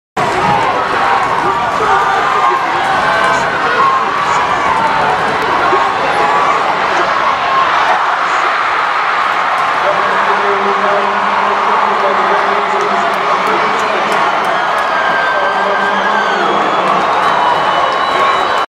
Waianae vs. Kahuku: Kaonohi Kaniho, interception - HHSAA Open Semifinal (2017)
Video by CJ Caraang.